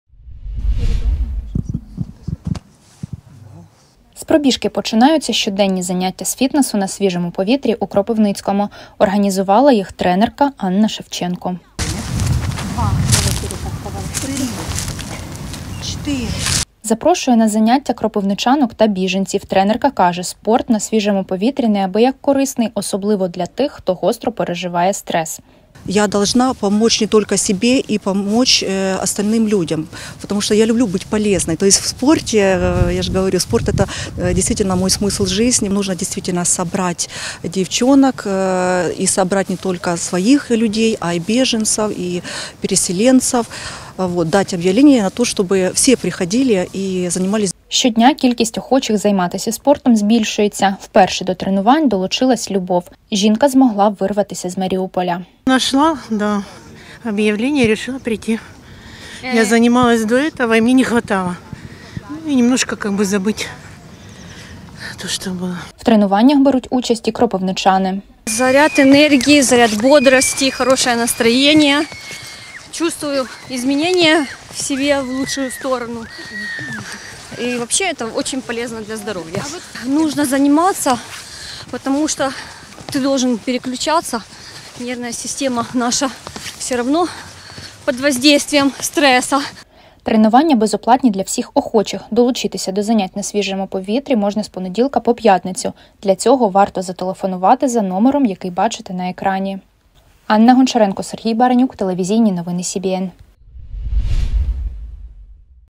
Безоплатні тренування з фітнесу на свіжому повітрі проводять у Кропивницькому | телеканал Вітер
З пробіжки починаються щоденні заняття з фітнесу на свіжому повітрі у Кропивницькому. Організувала їх тренерка Анна Шевченко. Запрошує на заняття кропивничанок та біженців. Щодня кількість охочих займатися спортом збільшується. Тренерка каже, спорт на свіжому повітрі неабияк корисний, особливо для тих, хто гостро переживає стрес.